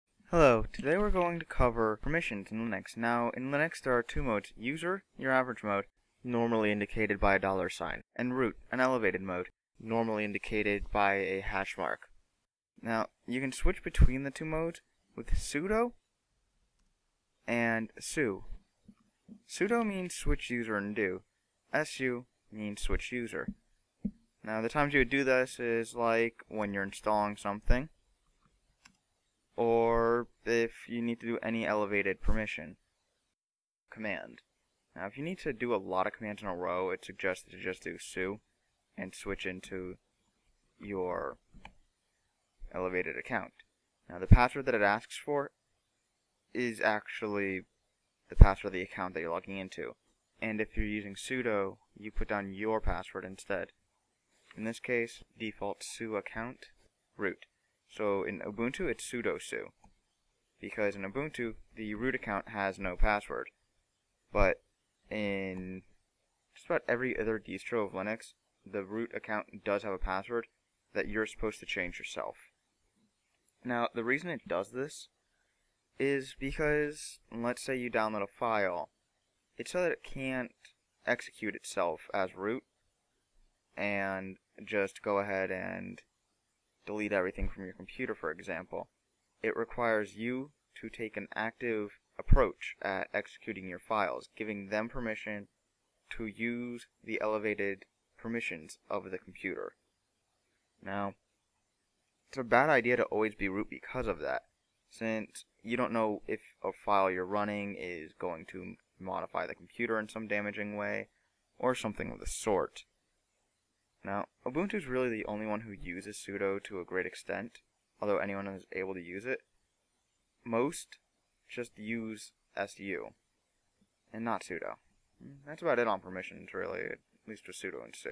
What is Sudo and Su in Linux
In this video we cover what sudo and su is, and why linux uses them instead of just allowing you to run anything like in windows.